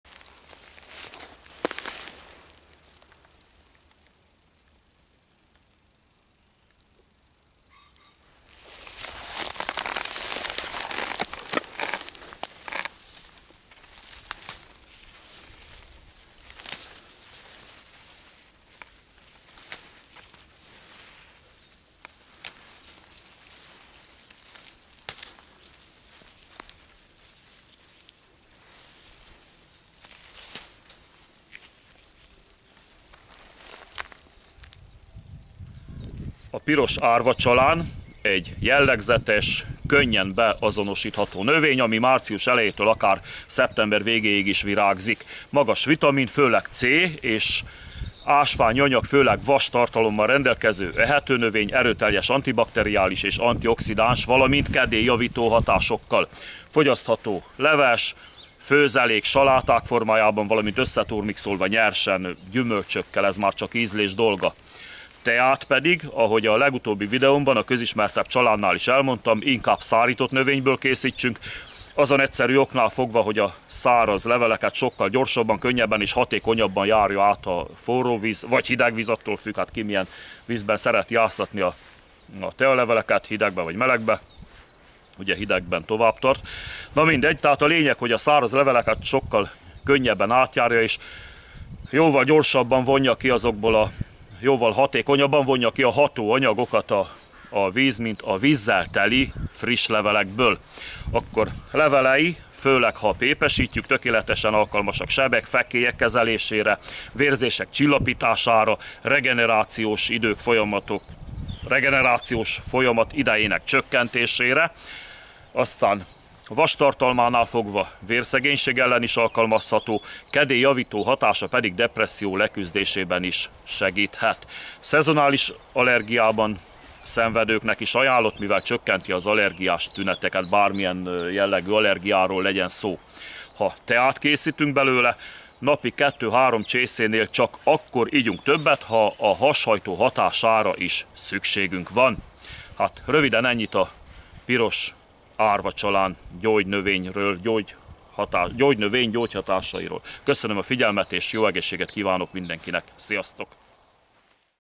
430. Piros árvacsalán pozitív hatásai az egészségre
A Piros árvacsalán rendkívül magas C-vitamin és vastartalommal rendelkező ehető gyógynövény. Alkalmas sebek, fekélyek kezelésére, vérzések csillapítására, valamint vérszegénység kezelésében és depresszió leküzdésére egyaránt. Nagyobb mennyiségben fogyasztva hashajtó hatással bír, illetve csökkenti az allergiás tüneteket.